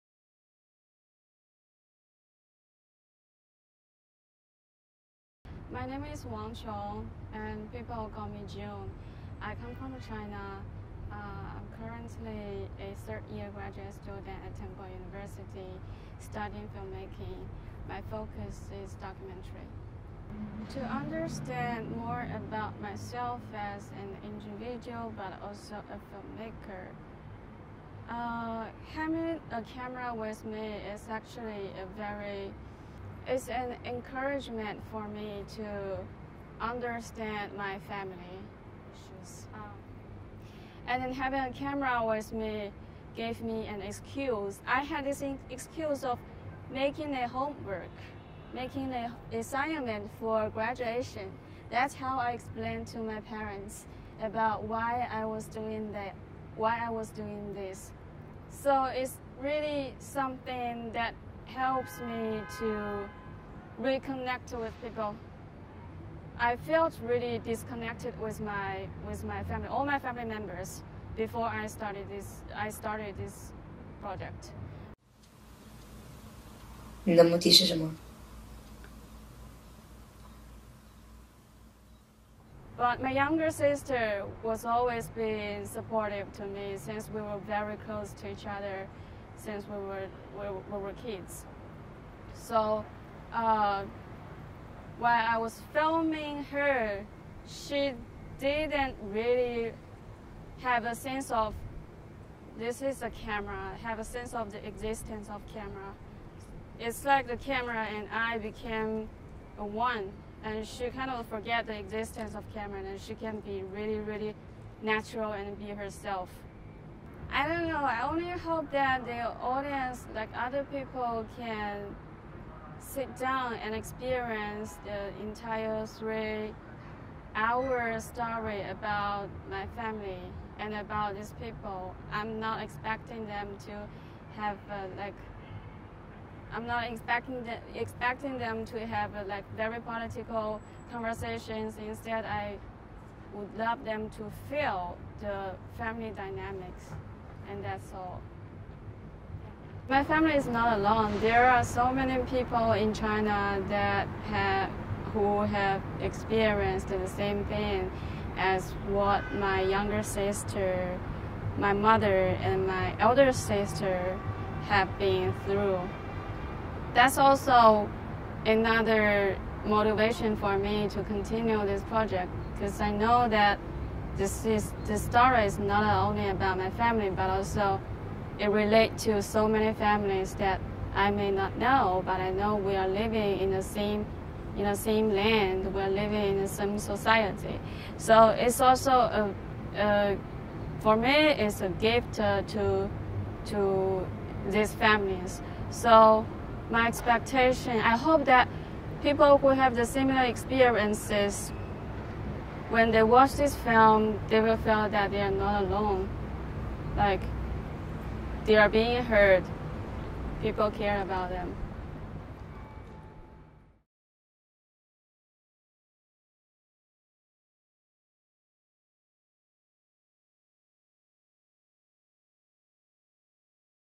Interview with MFA June Wang ('22) - All About My Sisters (short version)
Here is an edited version of our interview with Qiong “June” Wang (MFA ‘22) whose debut feature-length documentary ALL ABOUT MY SISTERS is receiving incredible attention this year. Filmed over seven years with her family in China, ALL ABOUT MY SISTERS follows Wang’s younger sister, who survived an abortion due to China’s one-child policy and other complex issues, and was later adopted by her uncle.

The film made its North American premiere at the 2021 New York Film Festival where the festival described it as “astonishing” and Wang as a “major new voice in nonfiction cinema.” Hyperallergic described it as one of the “don’t miss” films of the festival. Now, the film has been featured international stage at International Film Festival Rotterdam 2021 and The London Film Festival 2021, just to name a few.

This week, the film also premiered locally at the Philadelphia Film Festival!  We are so proud of Wang and honored to have her as a Temple FMA MFA student.